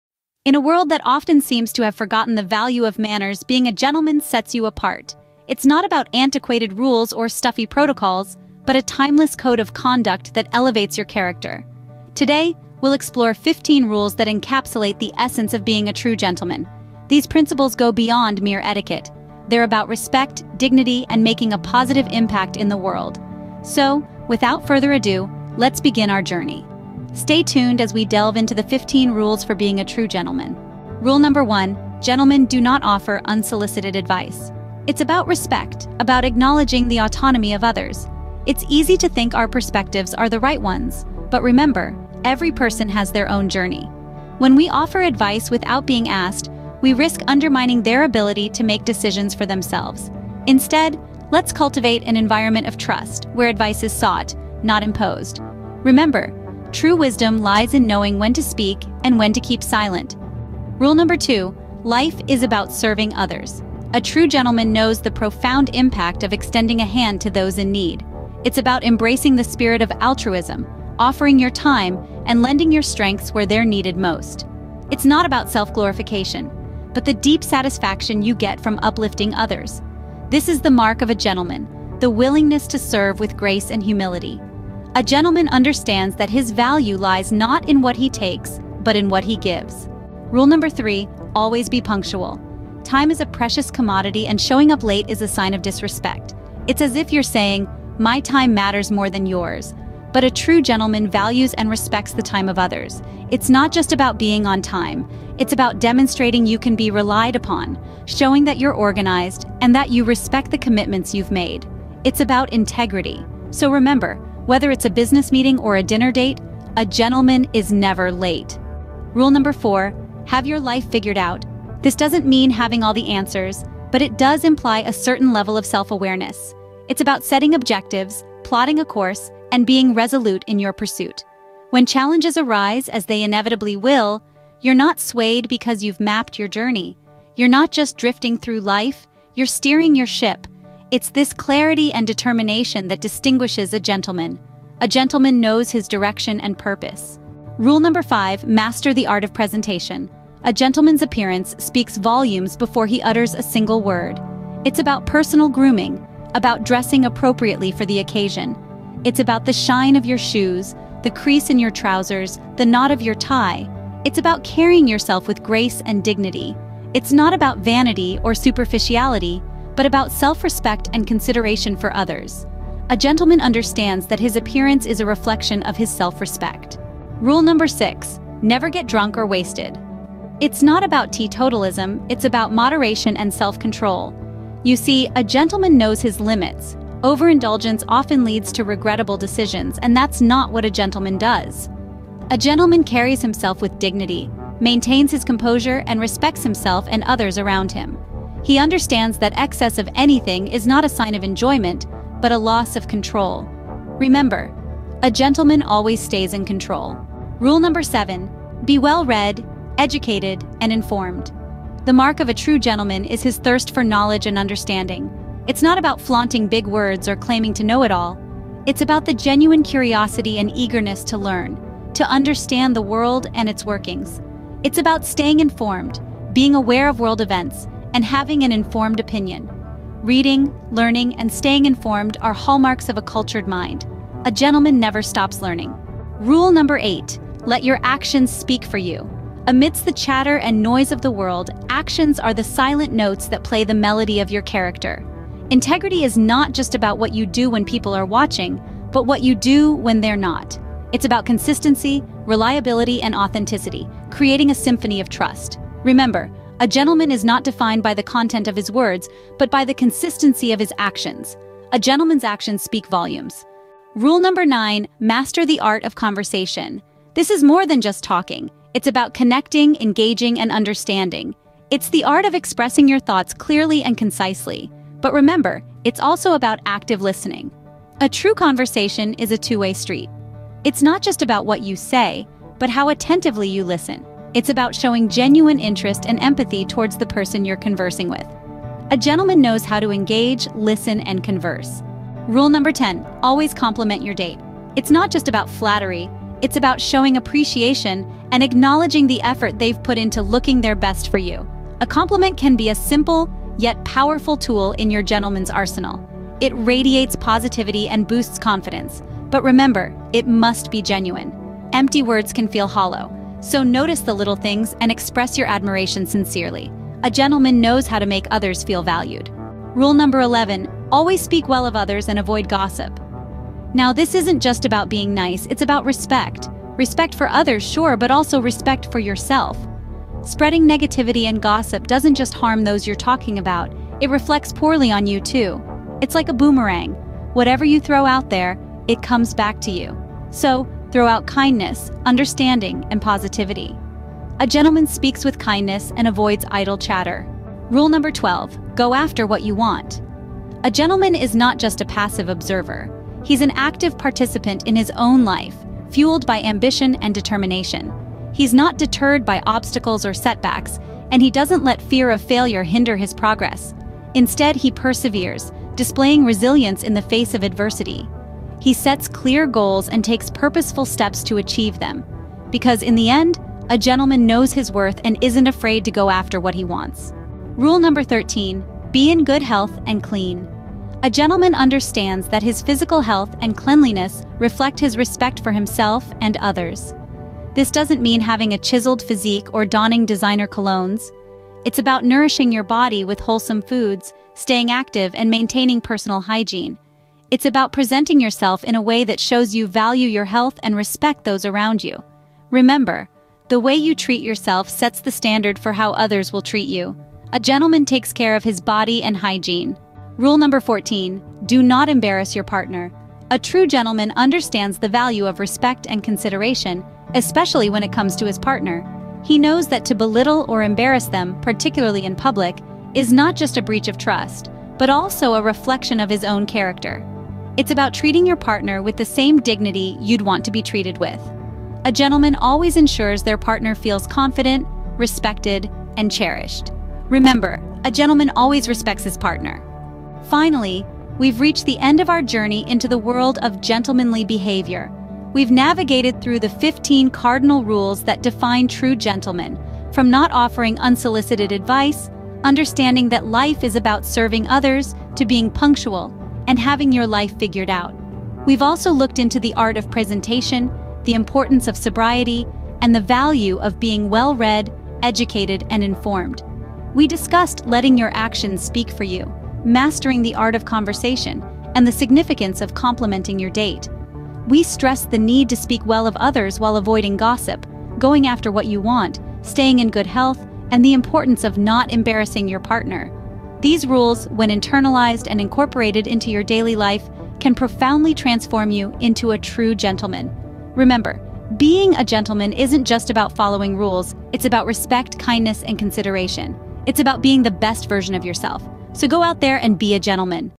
15 Unwritten Rules: The Gentleman's Guide
15 Unwritten Rules: The Gentleman's Guide

Unlock the secrets of gentlemanly conduct with our guide to the 15 Unwritten Rules every man should know. From timeless etiquette to modern-day manners, this video offers essential insights for navigating social situations with grace and charm. Join us on a journey through the nuances of gentlemanliness and elevate your personal style.